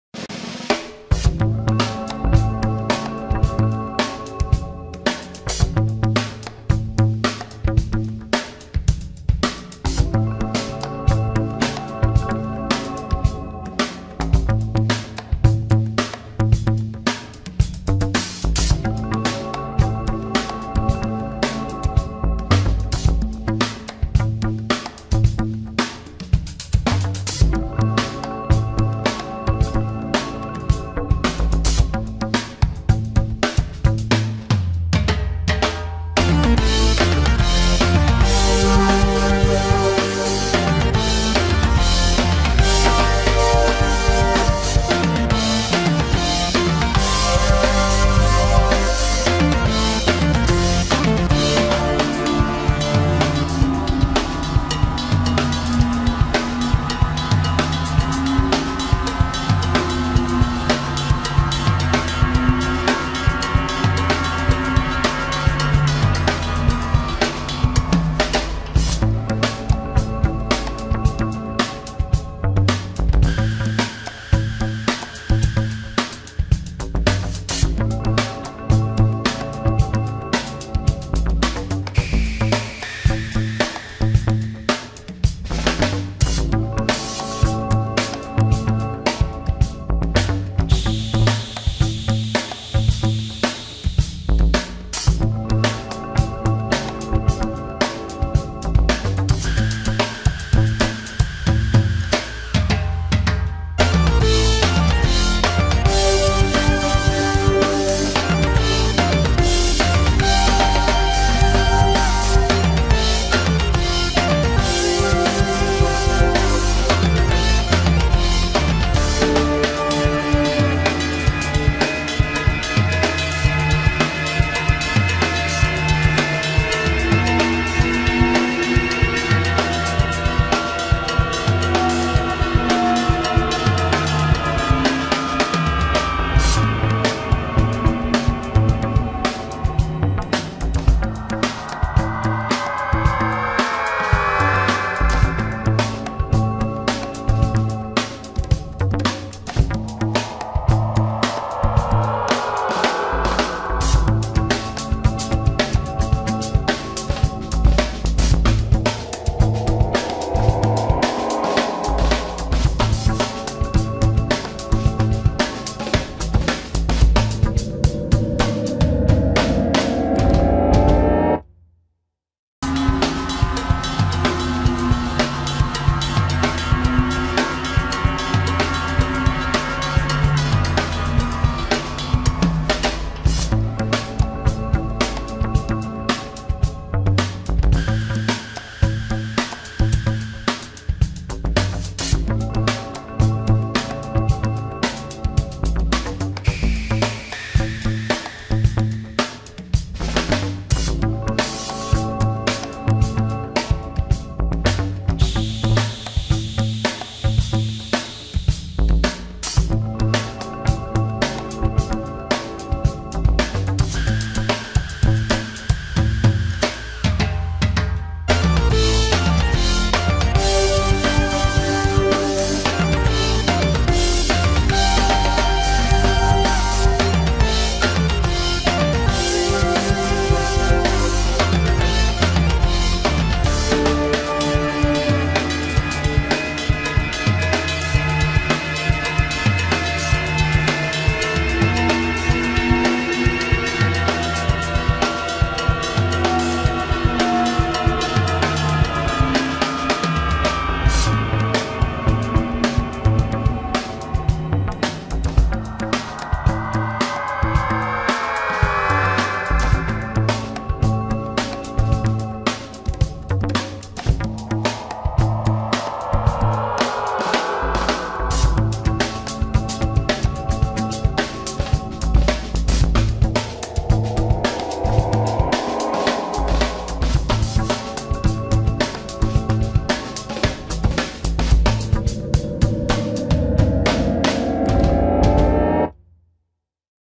EURUSD H1 FORECAST 3April 2022-My Strategy
Hello Traders
In This Video EURUSD Weekly Forex Forecast   By World of Forex

🟢This Video includes_  (EURUSD weekly market update)
🟢What is The Next Opportunity on EURUSD Market